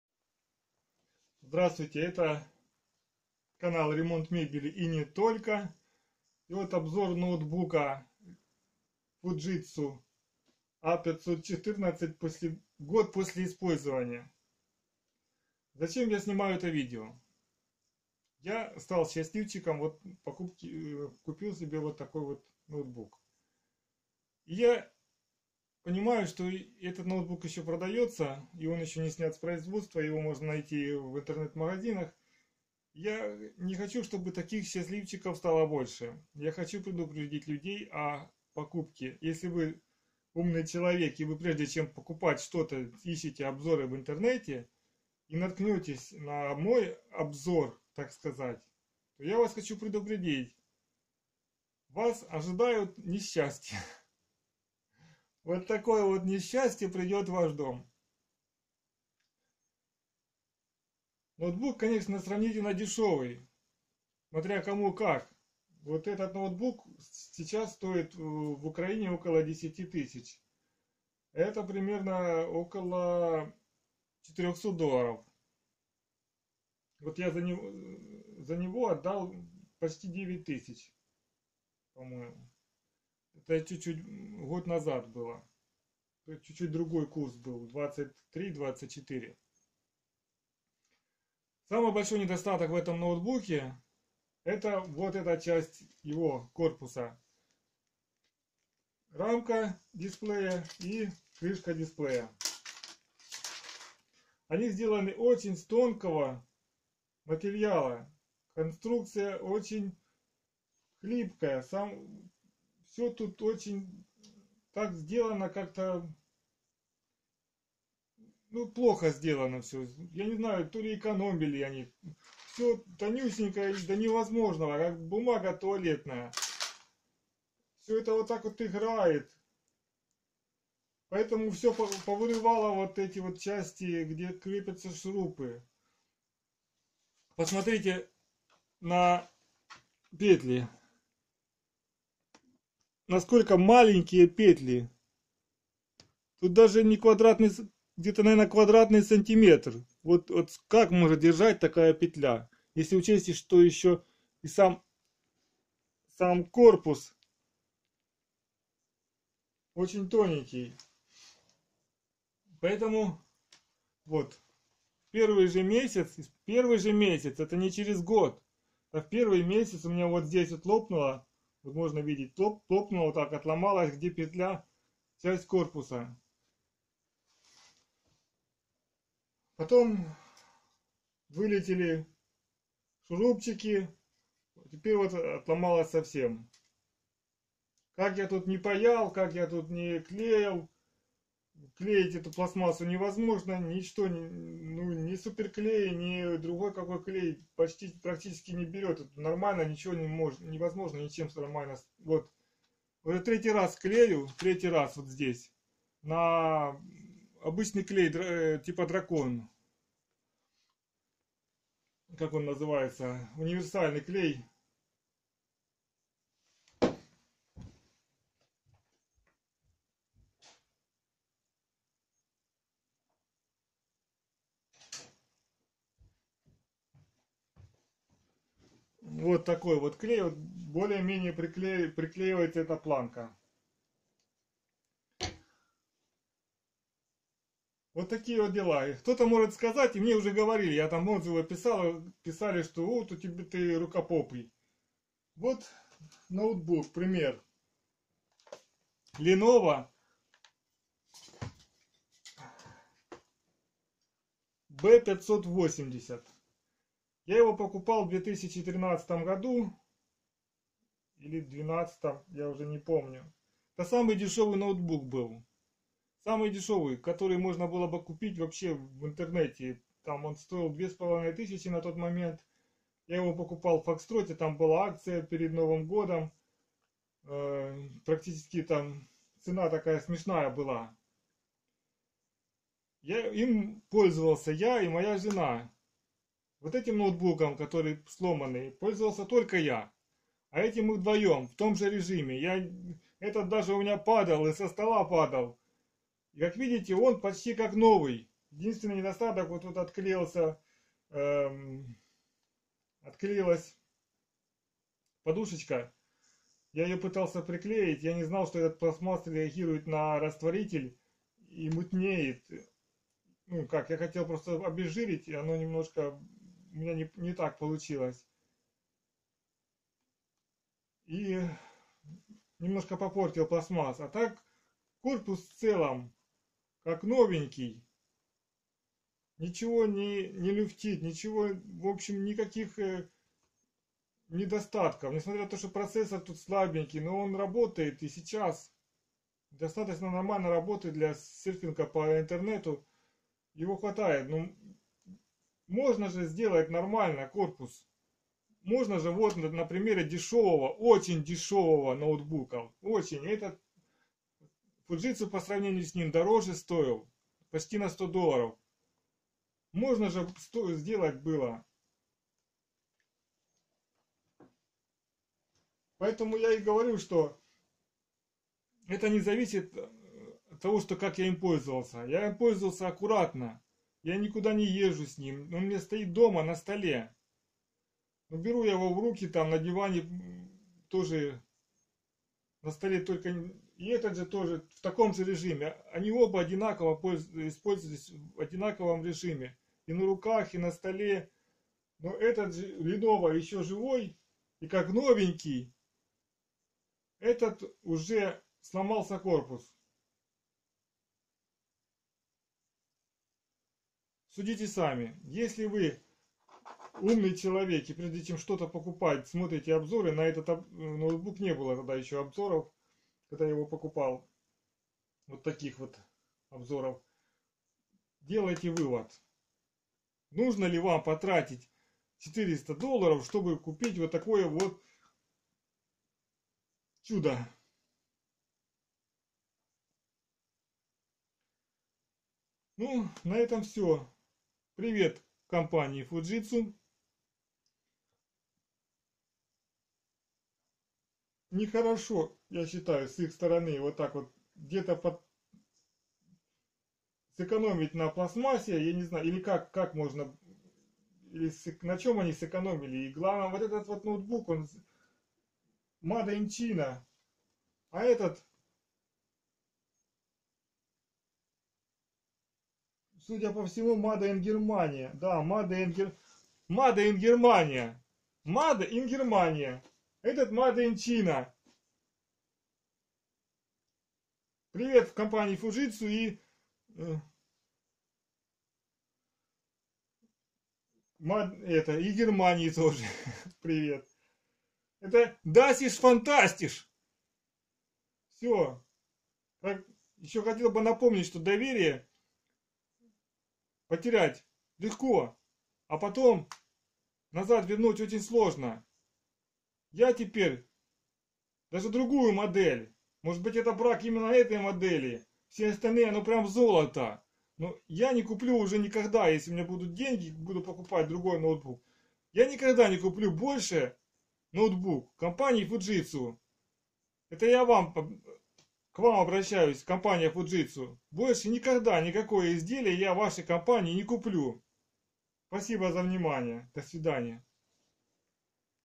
FUJITSU LIFEBOOK A514 обзор горький опыт использования.
FUJITSU LIFEBOOK A514  цены на крышку матрицы 73  ЕВРО!  рамка 37 ЕВРО!! вместе 110 ЕВРО!!!